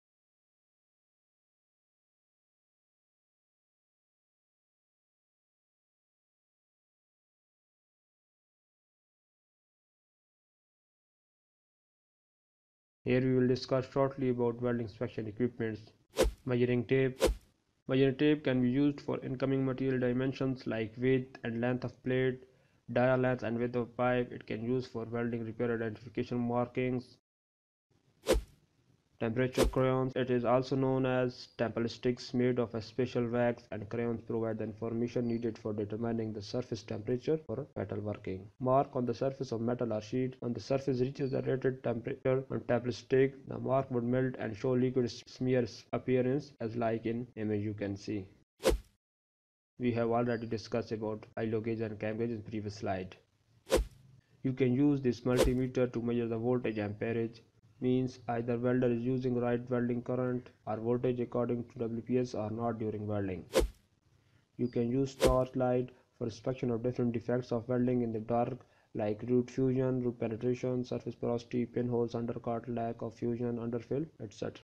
Welding Inspection Equipments Use By Welding Inspector.
In this video you will about different tools/equipments use by welding inspector